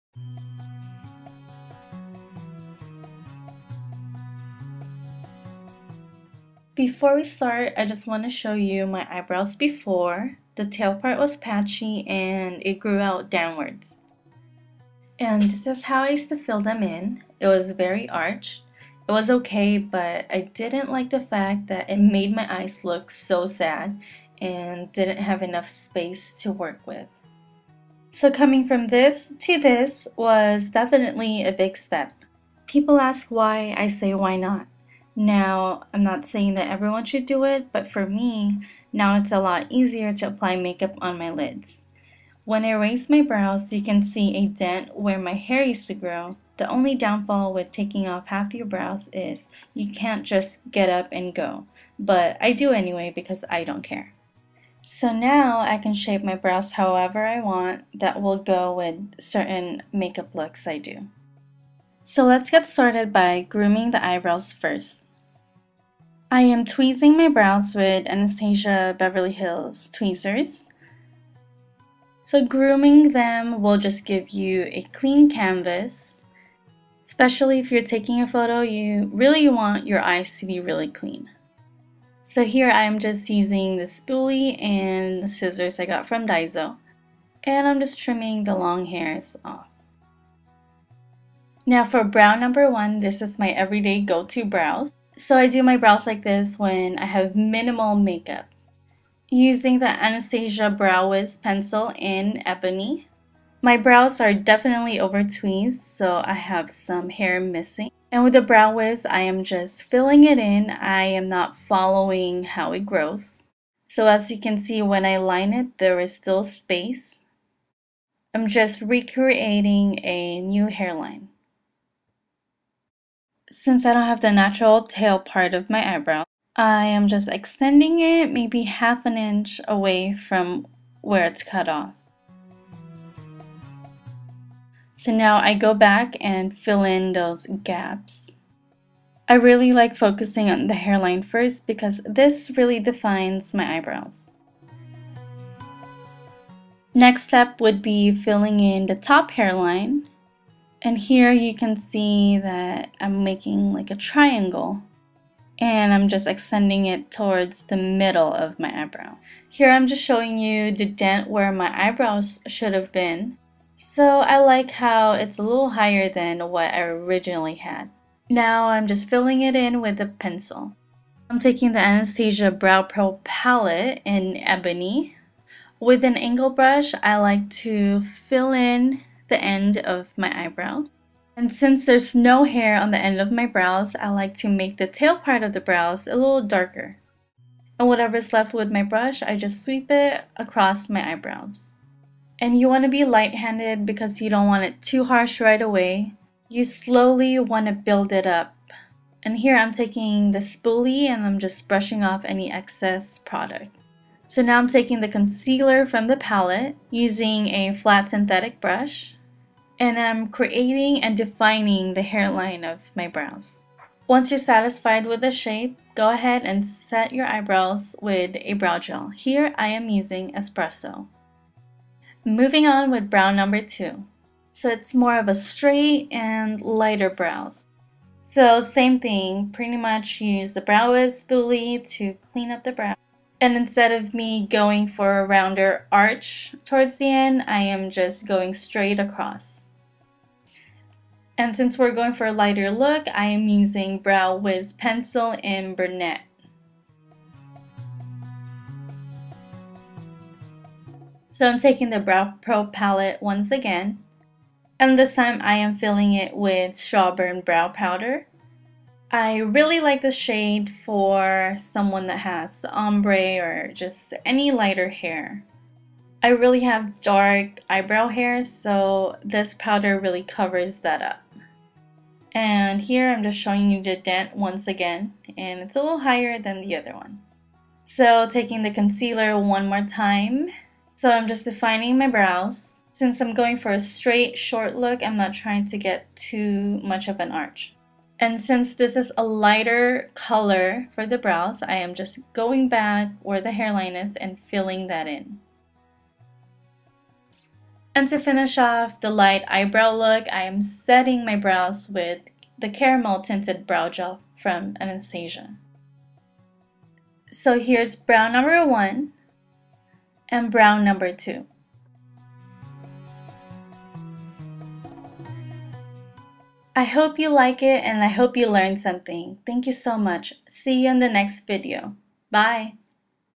NEW eyebrow tutorial ft. ANASTASIA by | FRITZIE TORRES
:::READ ME:::

hey everyone! this video is long over due :D

if you have been following me on instagram then you probably have seen a big transformation in my brows.. i like changing my brow shape depending on how i feel or what kind of look i am going for...my old brows made my eyes look droopy..now, i can shape my eyebrows however i want..the only down side with what i did, is that i cant just get up and go..but that doesnt stop me from going out without filling them in...

in this video you will see 2 new updated brow routine..
brow #1  is what i do most of the time
brow #2 is when i usually do a lot of dramatic eye makeup

products used

ANASTASIA BROW PRO PALETTE
BROW WIZ BRUNETTE AND EBONY
TINTED BROW GEL IN CARAMEL AND ESPRESSO